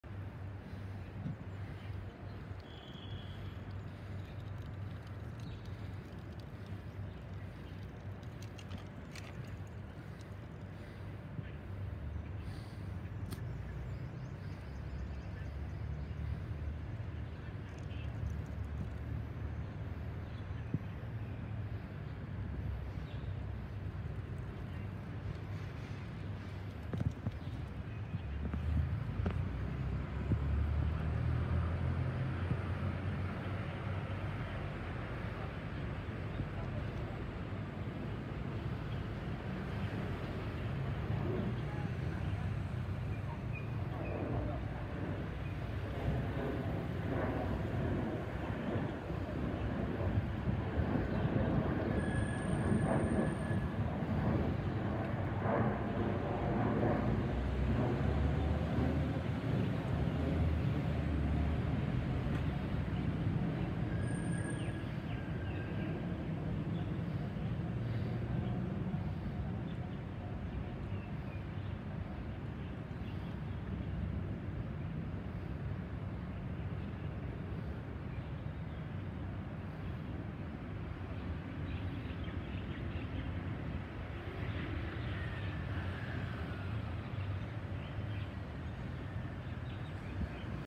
privatejet businessjet bizjet airplane aircraft mjets gulfstream glf5 HS-WEH flyover
MJets Gulfstream G-V (GLF5) HS-WEH flyover in Bangkok (Unknown flight number DMK-?)